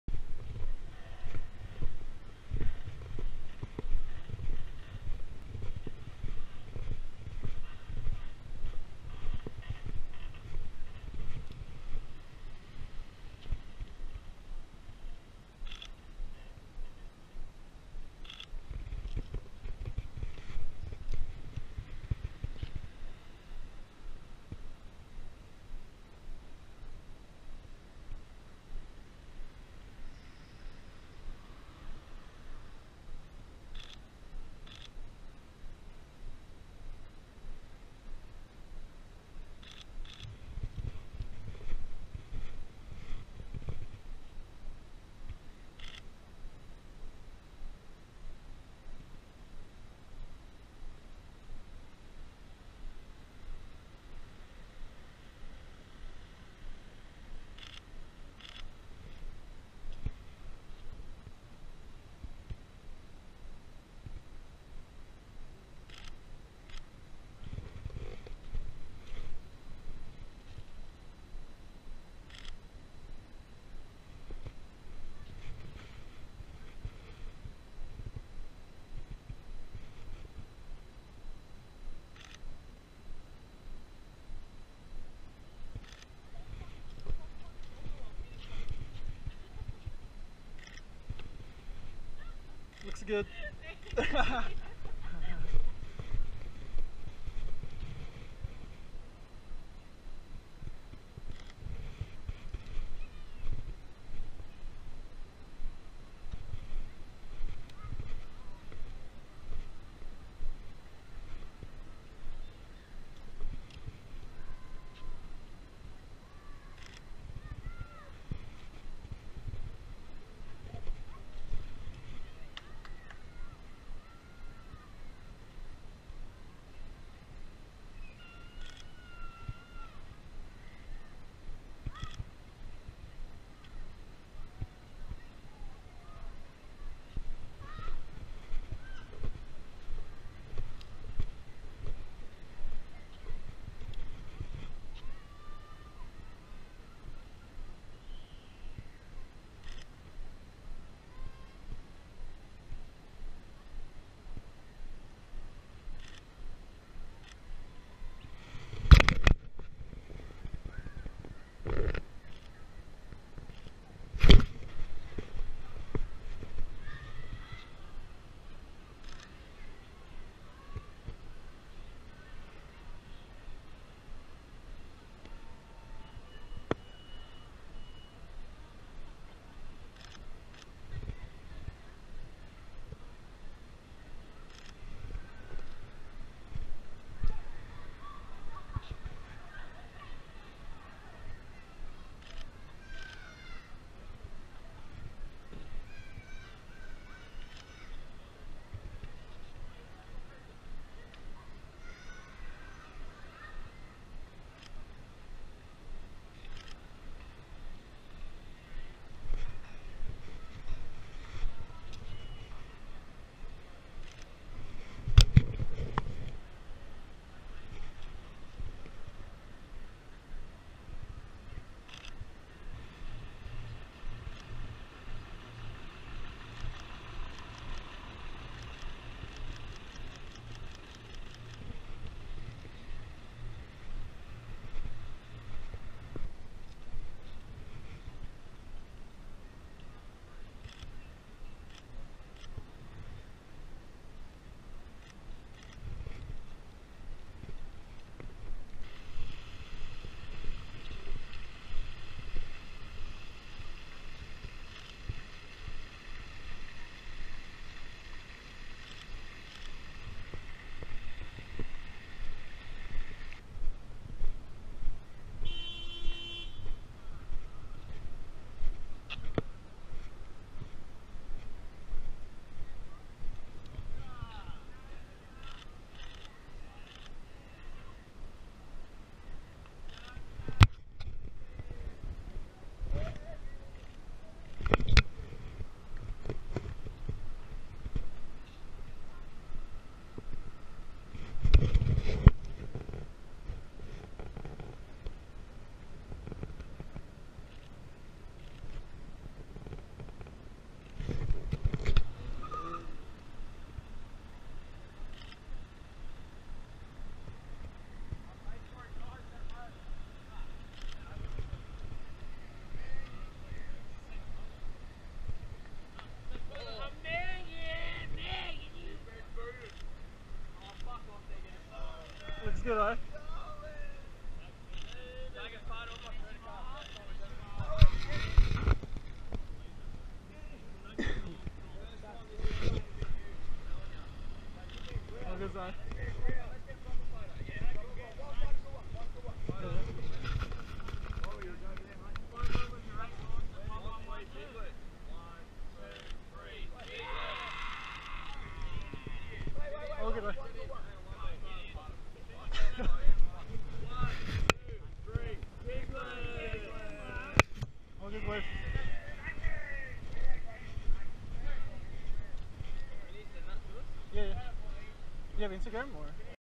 Soothing Street Photography in CAIRNS POV (85mm Sony F1.8)
Tonight I'm photographing the city of Cairns.

Camera Gear:
Sony A7iii
Sony 85mm f1.8
Sigma 35mm f.14